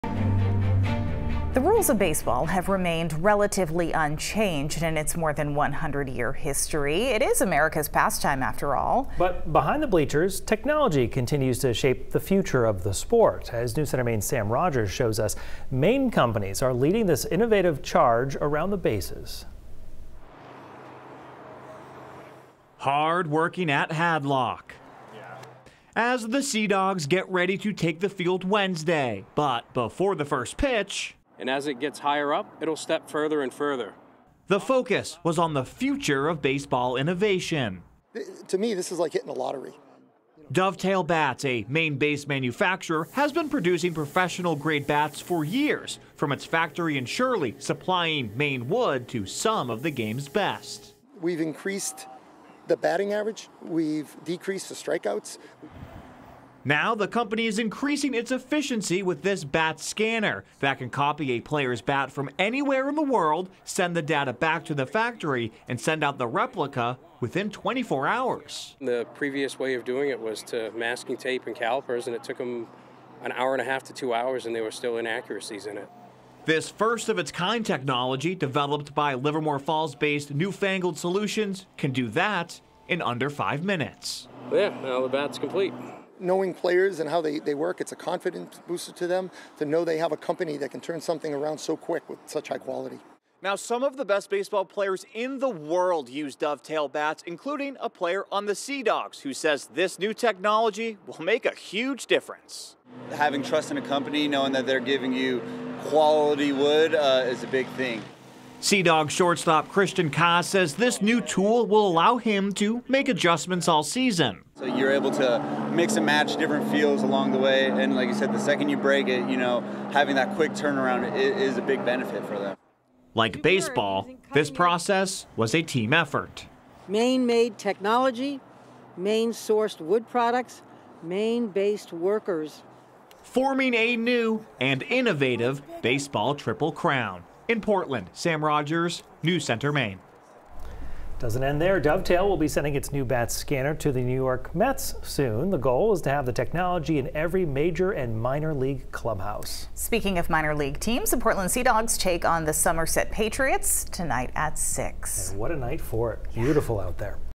Maine companies are reshaping the future of baseball
Dove Tail Bats unveiled its new bat scanner Wednesday. The technology can scan a player's bat and send the data back to quickly produce high-quality replicas.